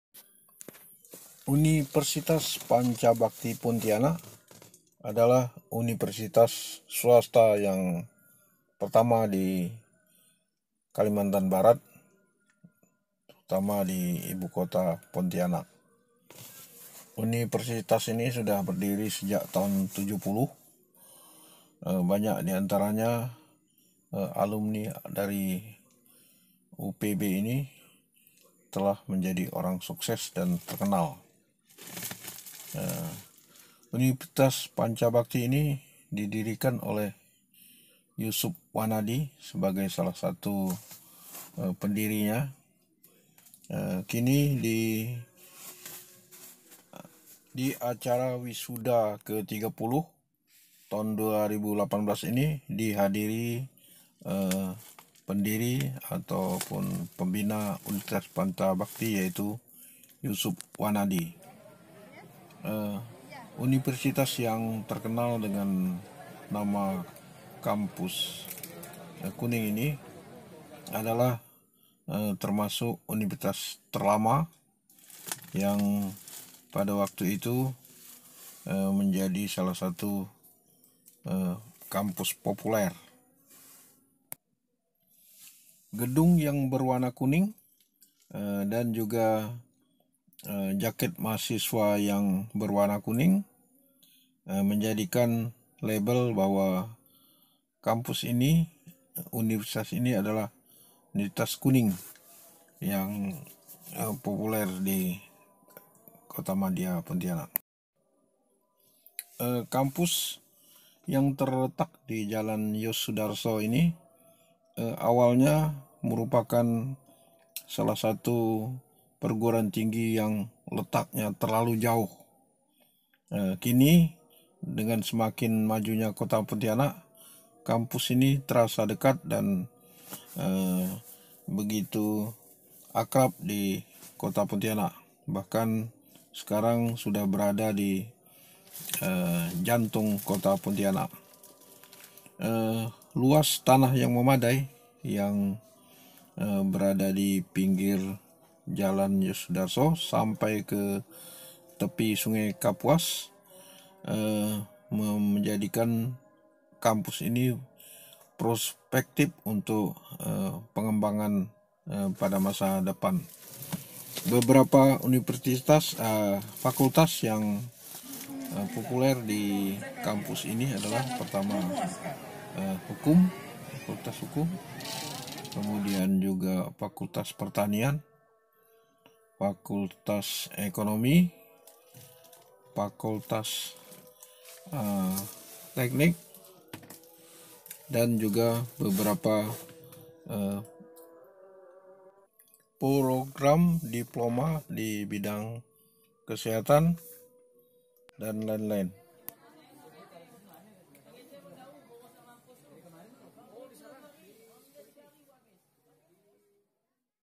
Yusuf Wanandi hadiri wisuda pancha Bhakti
Pendiri dan pembina universitas panca Bhakti Pontianak hadiri wisuda ke ,30 kampus kuning